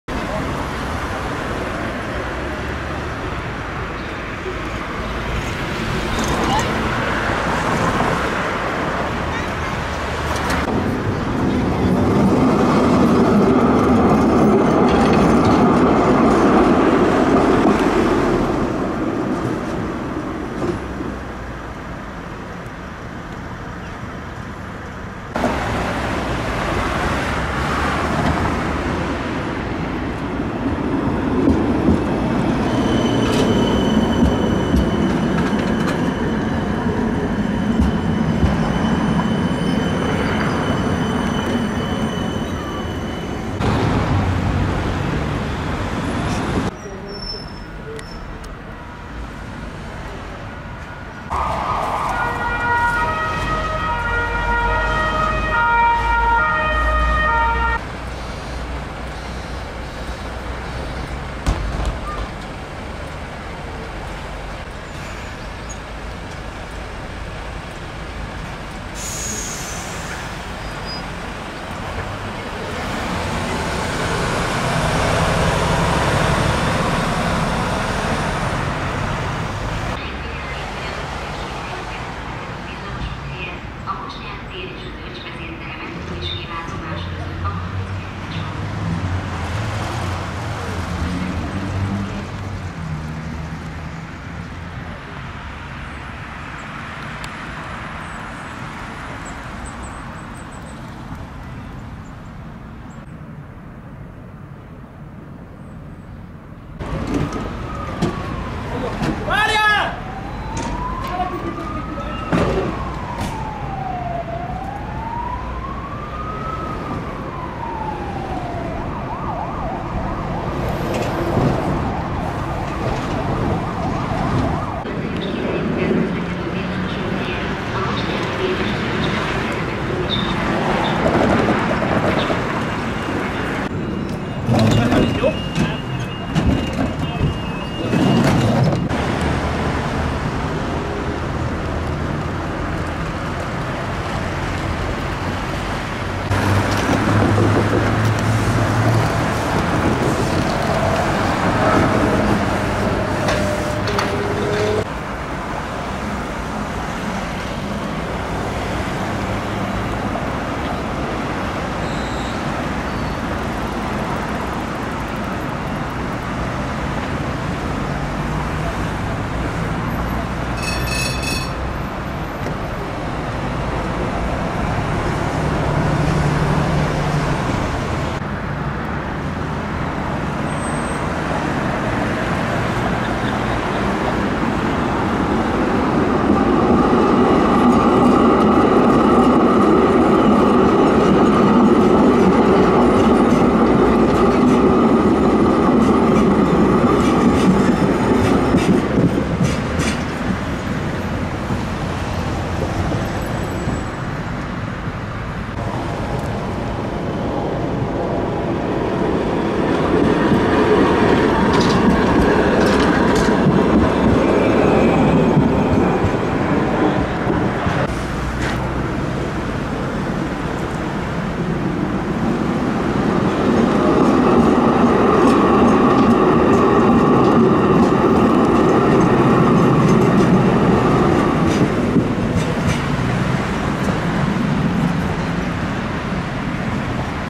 2022 Meghibásodott villamos a 3-as vonalon
Bankszámla:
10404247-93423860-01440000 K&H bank

Pongrátz Gergely téren üzemképtelen lett egy TW6000-es. Üzemzavar elhárító kiérkezésekor értem oda, készült pár snitt. Végül összecsatolták a mögötte lévő TW6000-essel és mikor már gurulni tudott a villamos csatoltan ment utasok nélkül a Bosnyák térnél lévő kocsiszínbe.